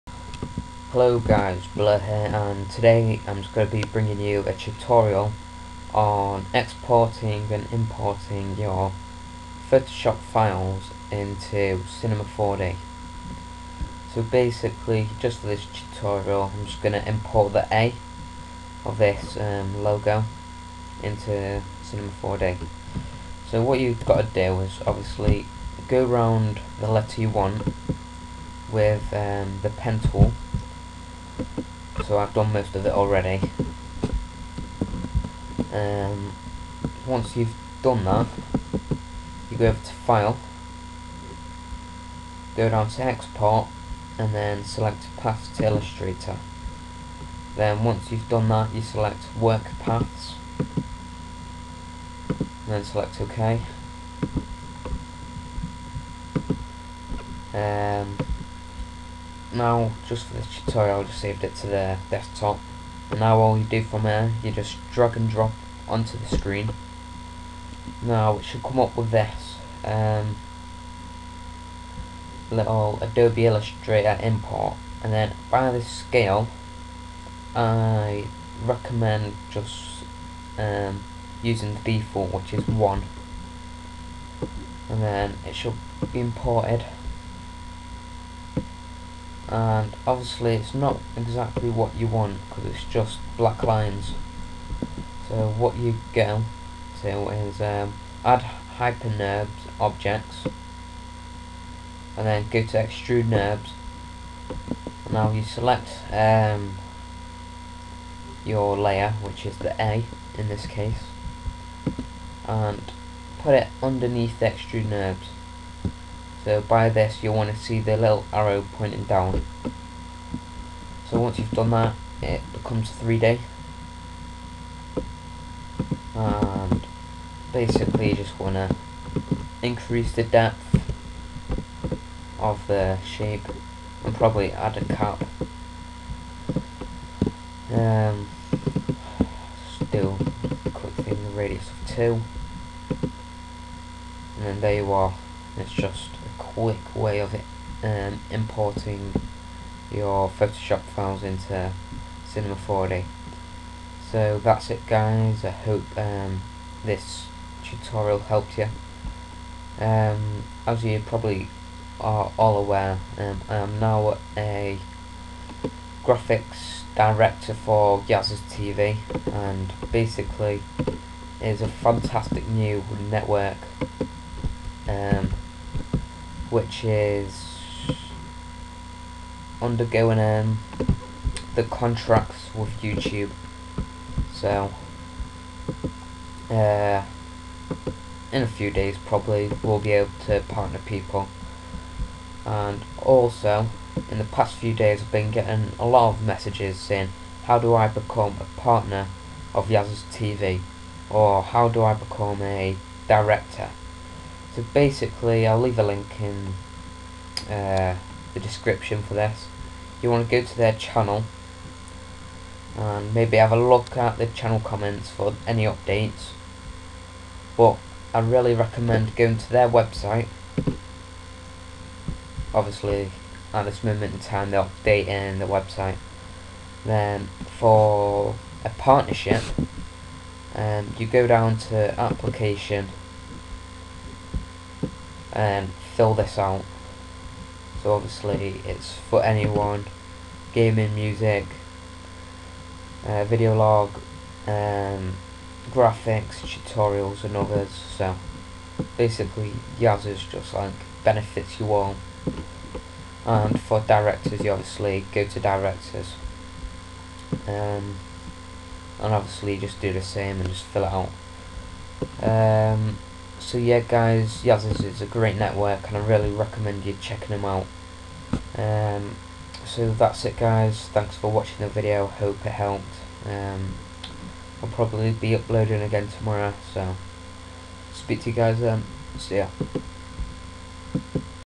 How to import file from Photoshop into Cinema4D
'How to import file from Photoshop into Cinema4D' - I hope this tutorial helped you and please check out YazursTV